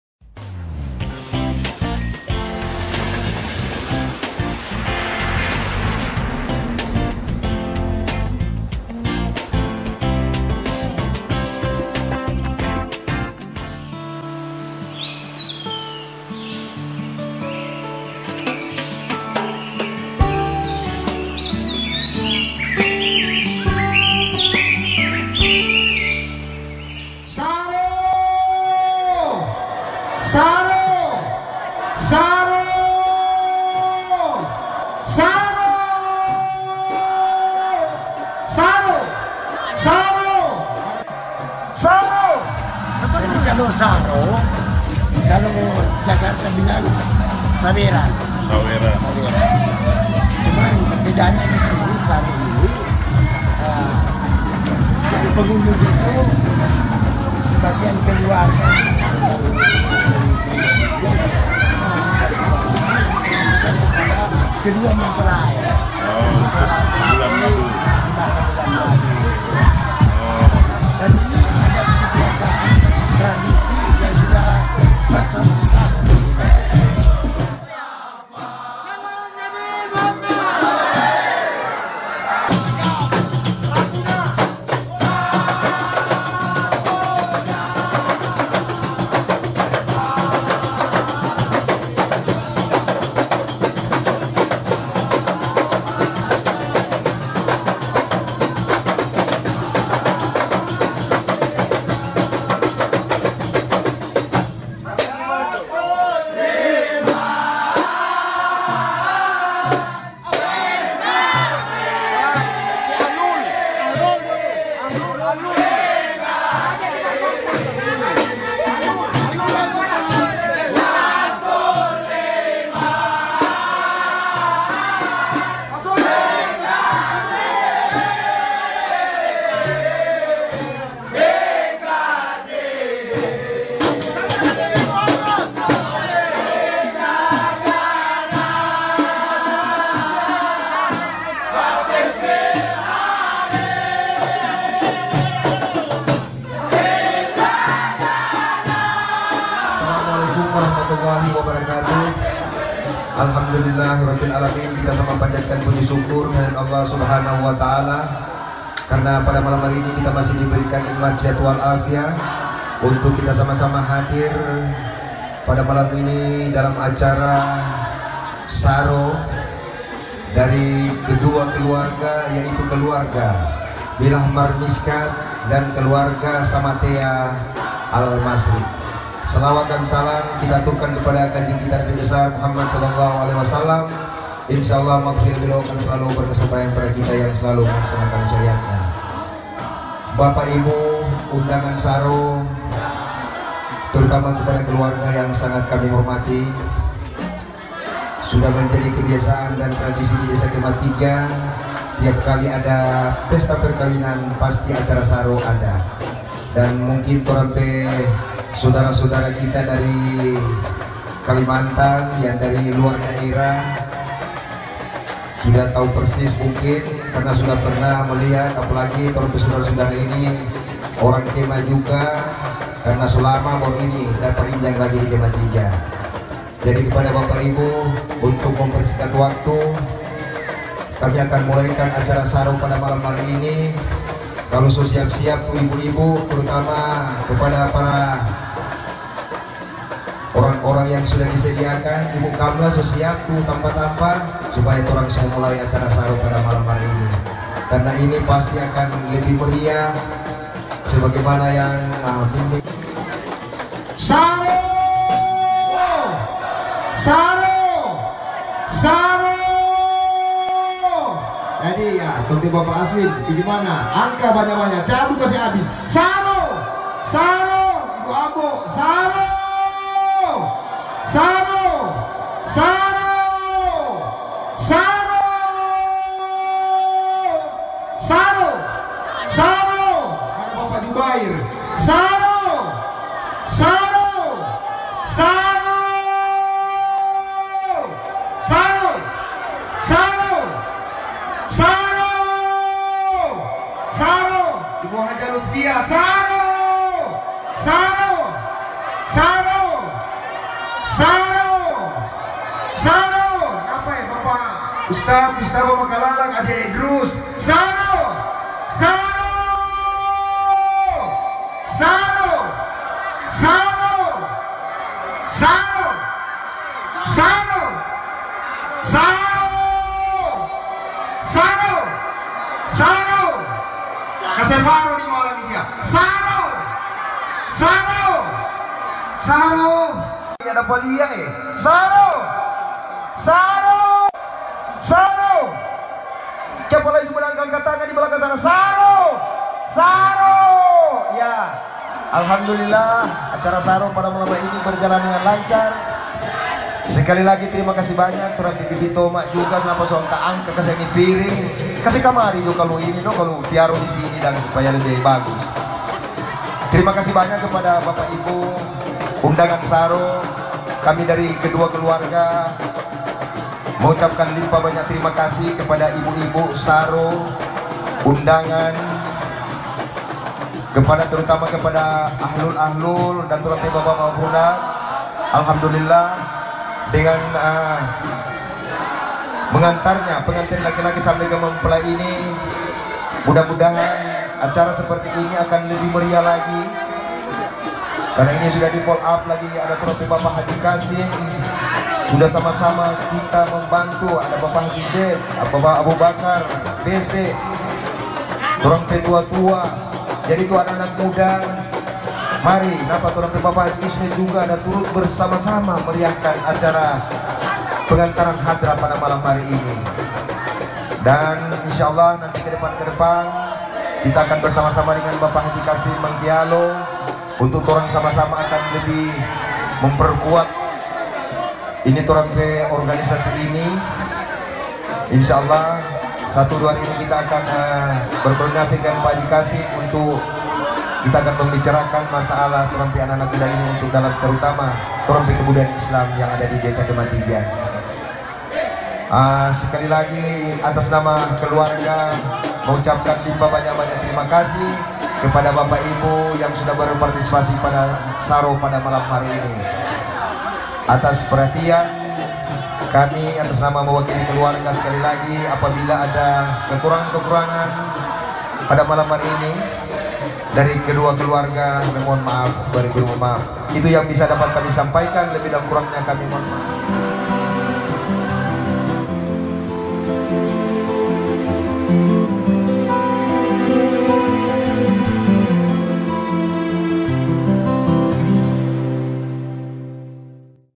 SARO ADAT PENGANTIN KEMA III
SARO adalah salah satu rangkaian acara adat perkawinan masyarakat yang mendiami Kampung tertua di Minahasa Utara.
"Saro - saro - saro"  terdengar teriakan pembawa acara saro, lalu tamu saro yang disediakan meja memasukan uang ke dalam pring.   Demikian pula masyarakat umum memasukan uang ke kotak-kotak yang dibawa jalan oleg Petugas Saro.
Saro itu seperti sawer, tetapi bedanya uang ini diberikan sebagai bekal awal kepada kedua mempelai untuk berbulan madu.
Saro itu adalah pemberian secara ikhlas dari para undangan Saro dan pengunjung lainnya.   Pembawa Acara sangat berperan penting dalam pelaksanaan Saro, selain harus menguasai siapa saja yang hadir di meja tamu saro, juga harus mengauasi psikologis layaknya sebuah lelang, yang emosi bisa saja menguras dompet untuk acara saro ini.
Acara saro biasa dilaksanakan malam setelah bersanding sebelum acara muda mudi dilaksanakan.   Mempelai prIa diarak dengan tetabuhan Hadrah dan barisan Rodat, yang sangat meriah,